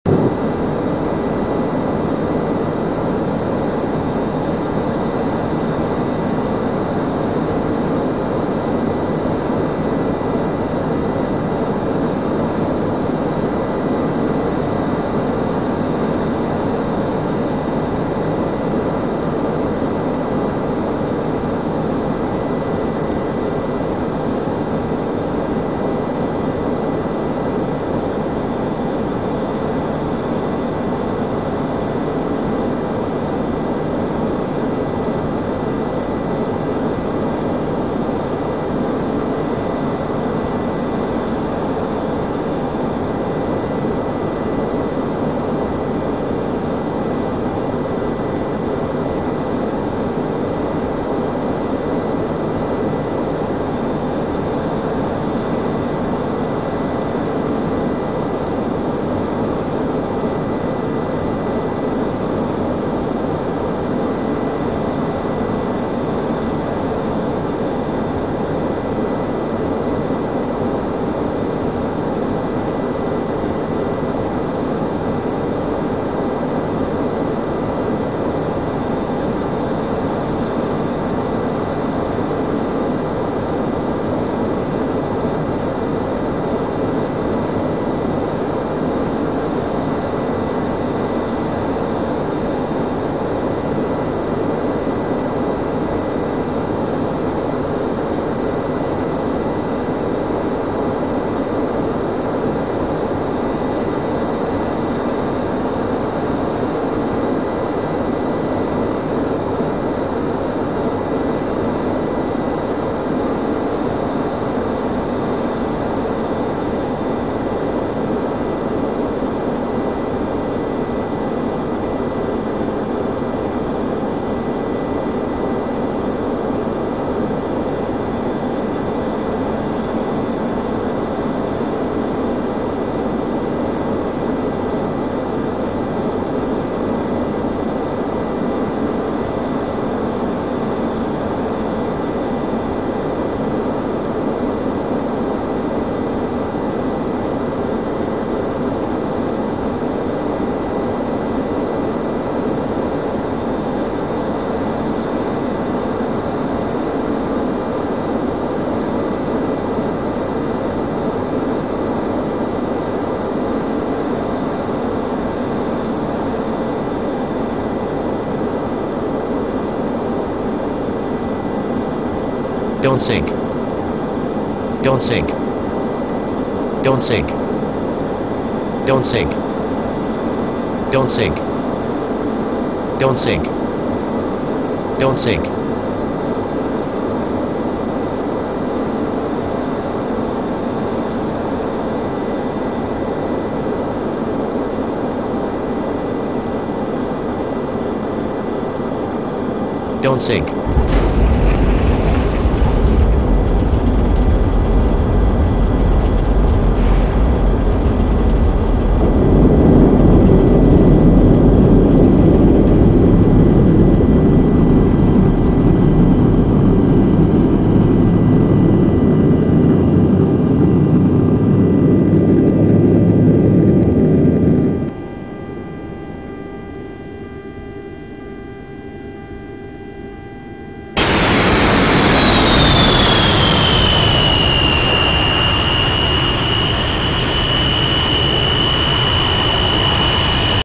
Landing on Pantelleria by 026
Approaching and landing on Pantelleria , with real weather condition , 30 -35 gusting from N , 'couse wind gust short and heavy very havy touch down.